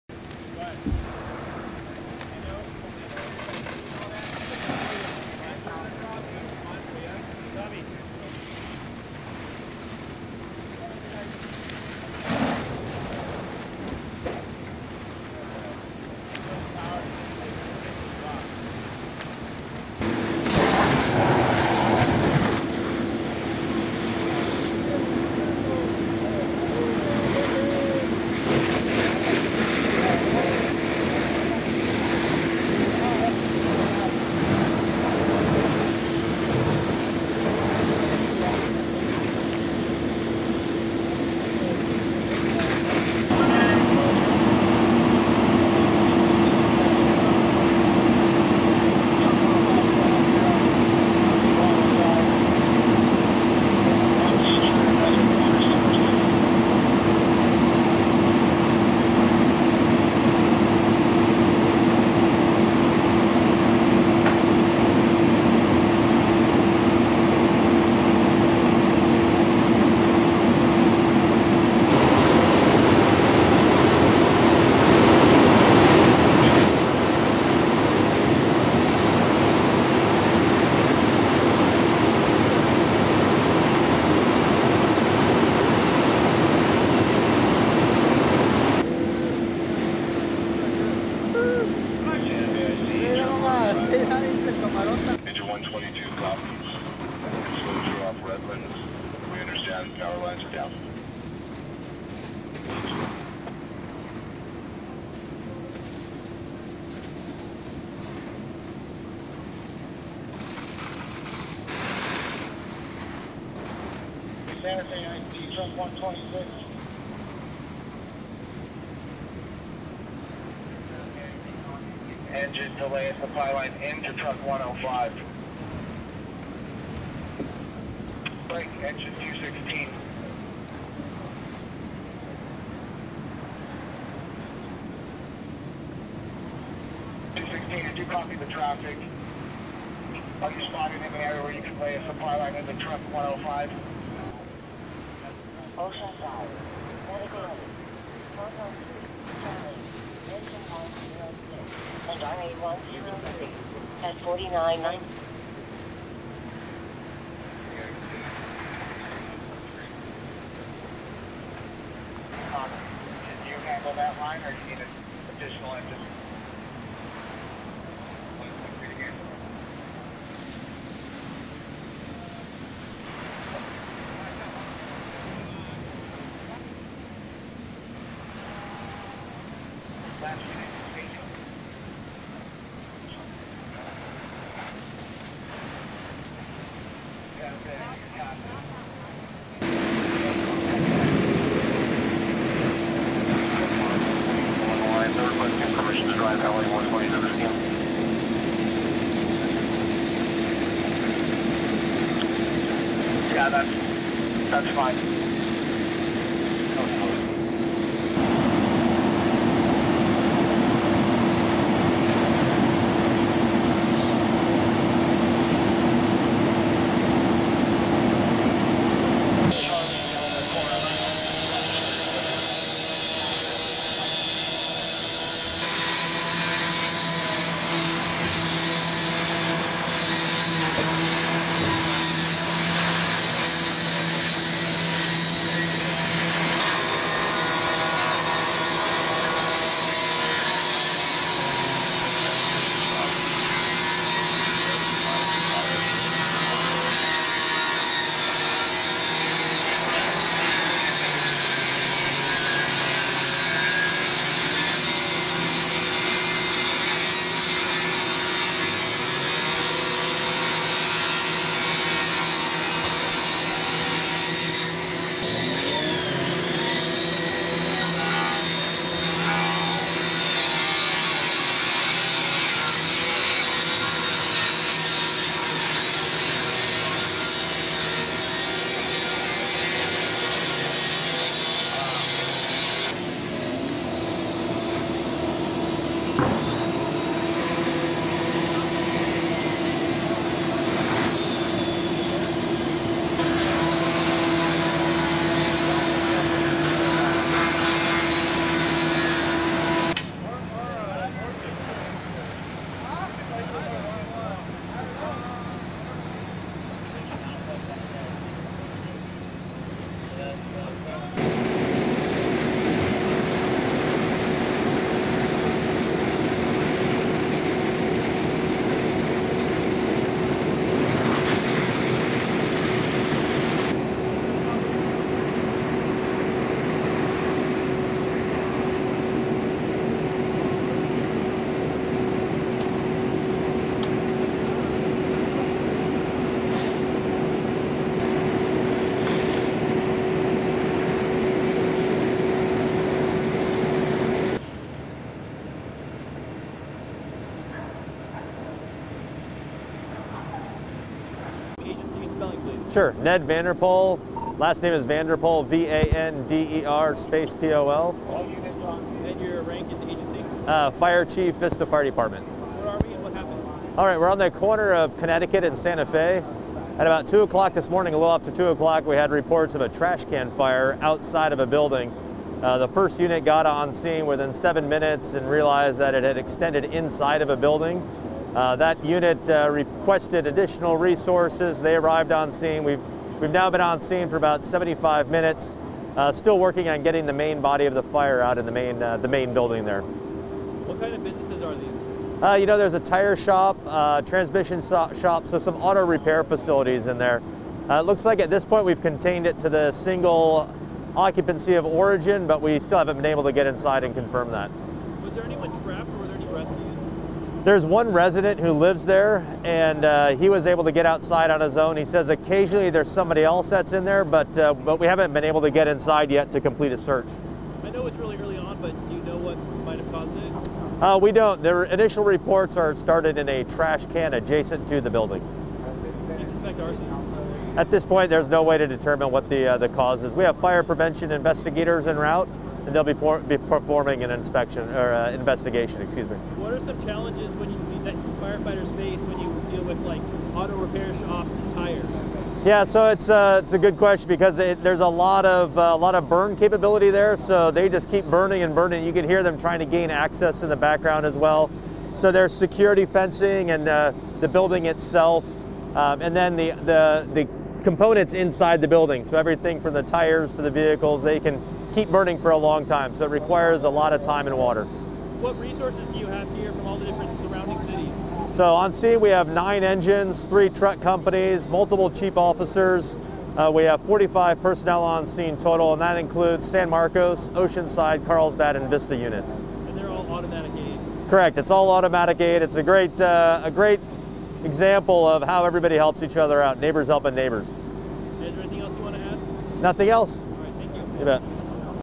Large 2nd Alarm Fire Heavily Damages Four Buildings | Vista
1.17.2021 | 2:07 AM | VISTA (CNS) - Four businesses were seriously damaged Sunday in a possible arson fire and residents of a nearby apartment complex had to be evacuated.

Around 2 a.m., deputies from the Vista Sheriff's Station discovered a fire in the alley behind the 400 block of North Santa Fe Avenue, said Sgt. A. W. Moses of the San Diego County Sheriff's Department.

The fire had quickly spread to at least four nearby businesses, Moses said.

Firefighters from the Vista Fire Department, Carlsbad Fire Department and Oceanside Fire Department were able to control and put out the fire, the sergeant said.

The fire appears to be suspicious, he said.

Smoke from the fire forced families to evacuate a nearby apartment complex, according to the San Diego Union-Tribune.

Detectives from the Sheriff's Bomb-Arson unit were handling the investigation.